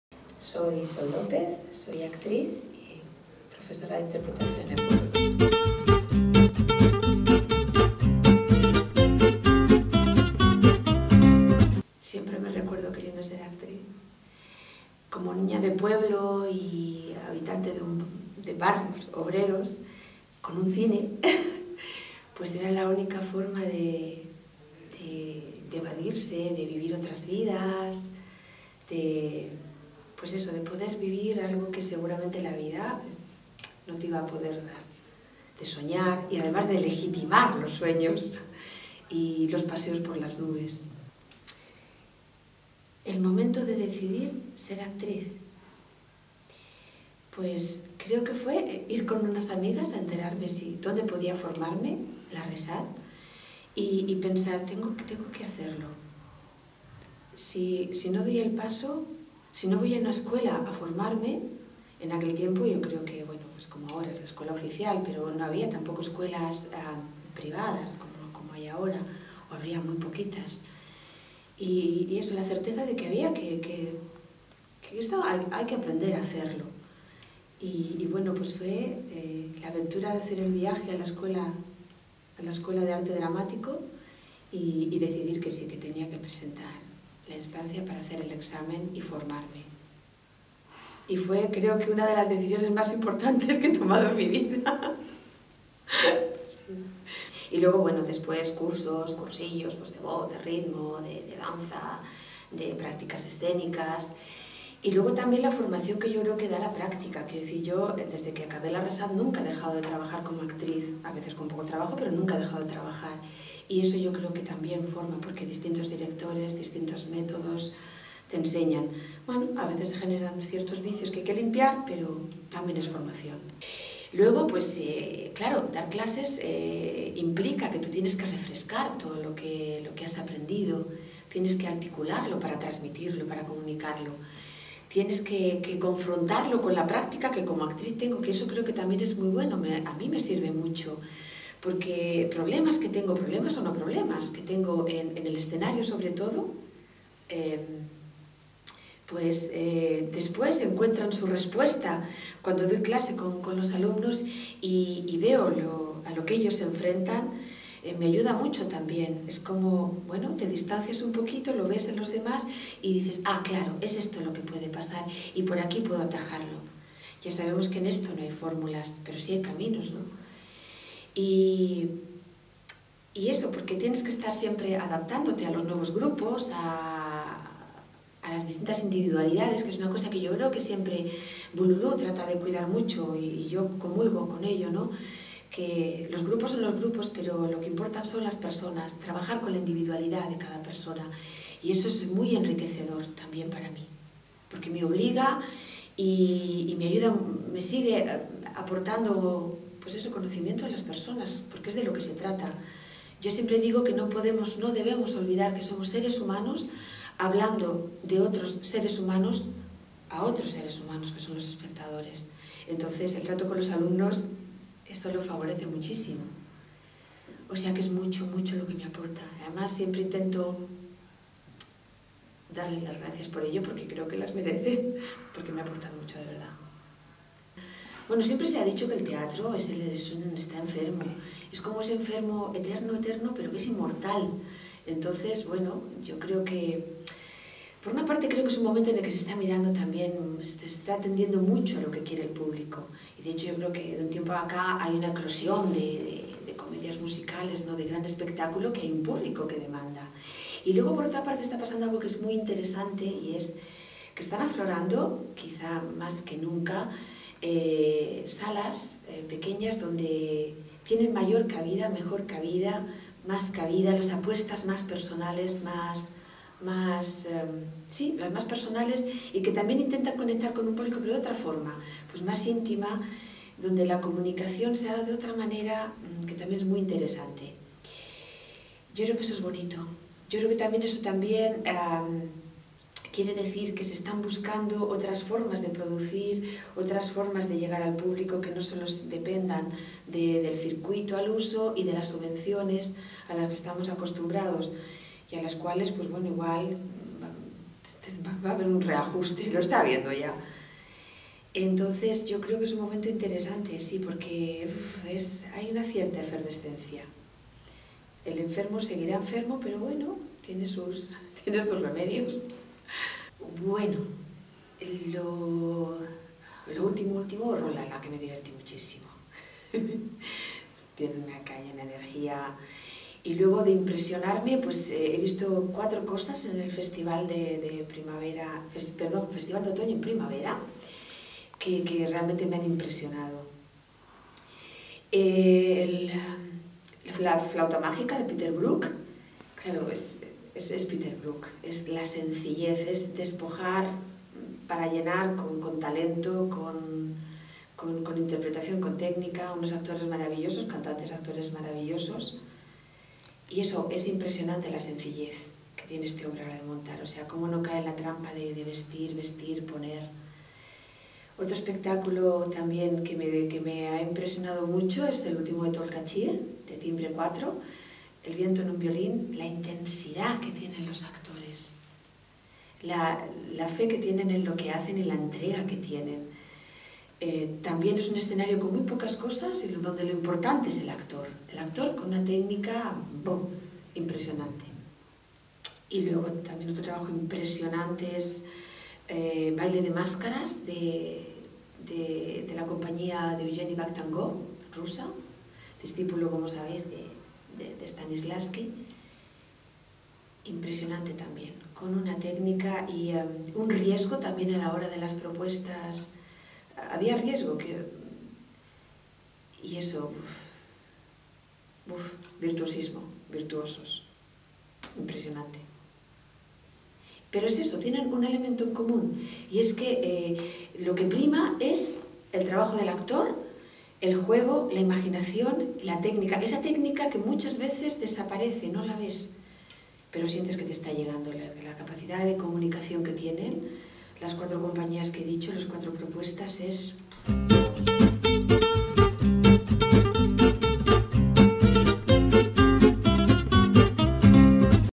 Entrevista Sol López
Entrevista con Sol López, actriz y profesora de interpretación en la escuela de teatro Bululú 2120, en Madrid.